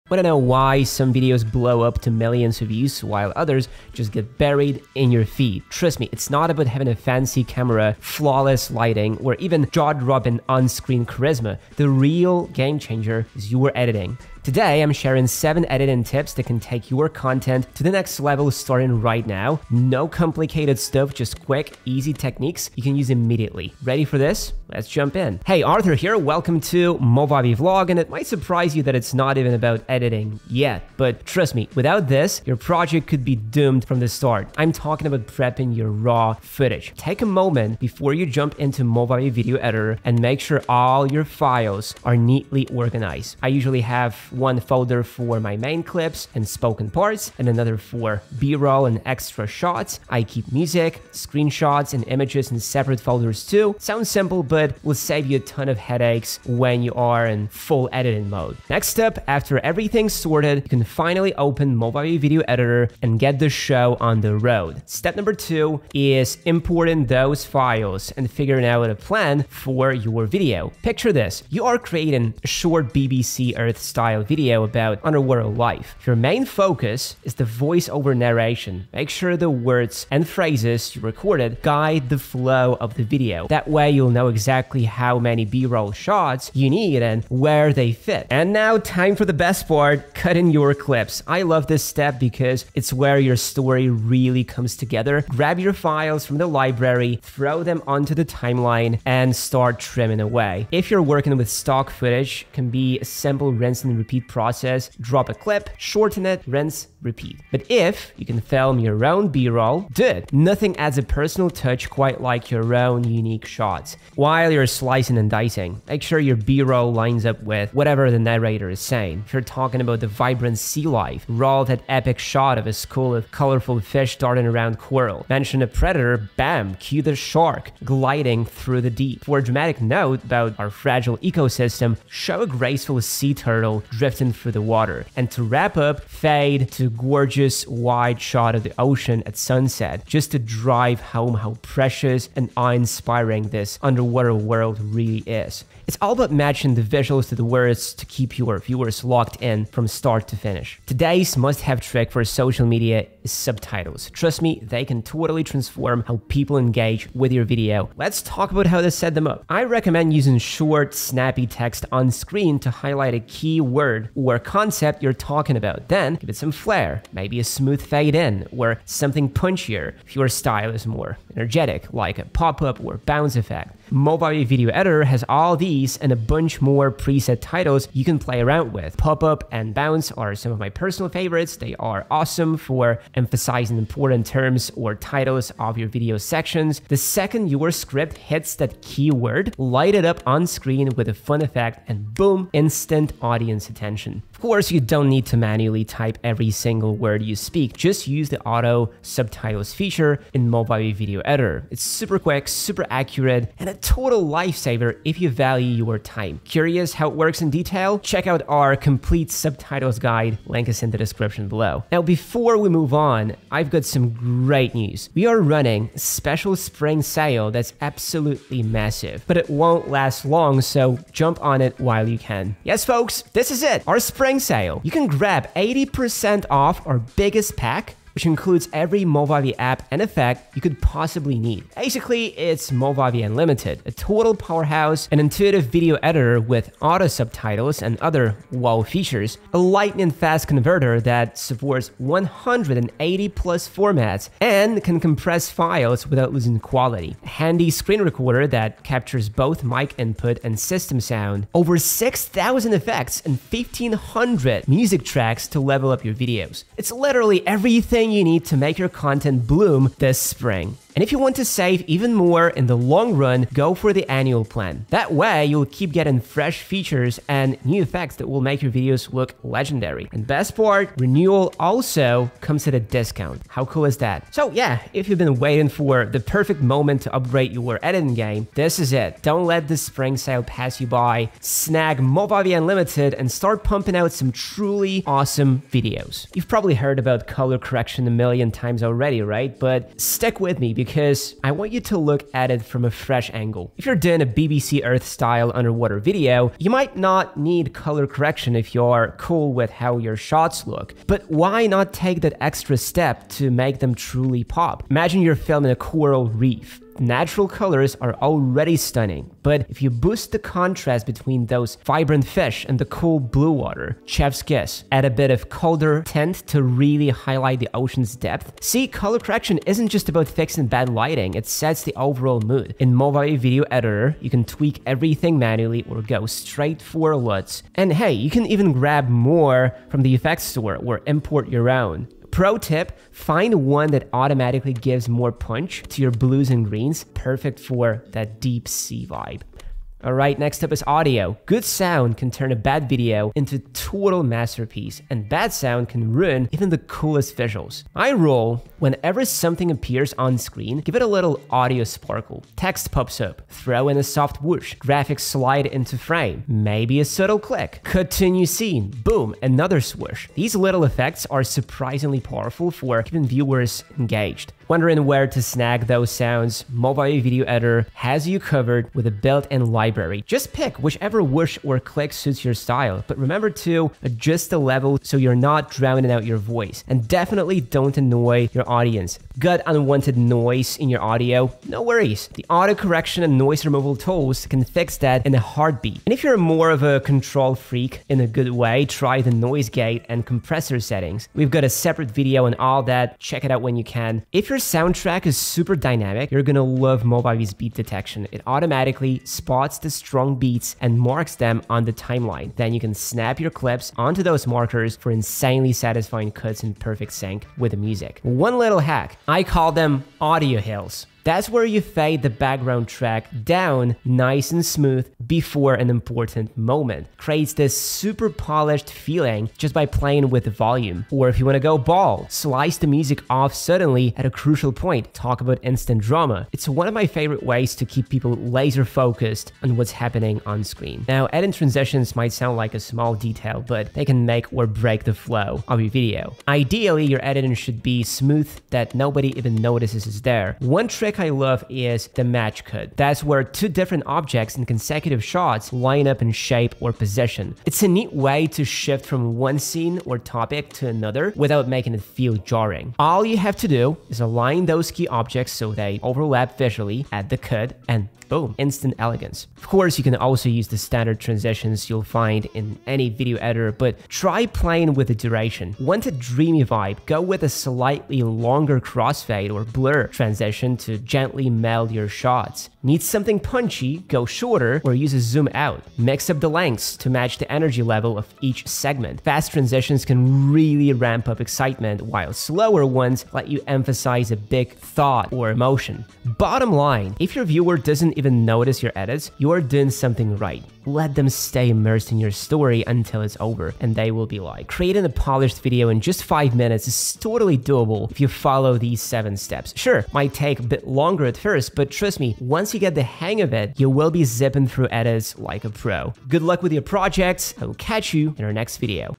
5-Minute Video Editing — A Guide That Actually Works | Movavi Video Editor
Want to edit videos but it all feels too complicated? This video is a clear, step-by-step guide to creating a full edit in just 5 minutes.
No experience, no heavy software, no stress. Perfect for beginners — whether it's for YouTube, social media, school, or your blog.

Give it a try — video editing can actually be easy when you know where to start.

Our Reddit community is on fire right now! and say hello!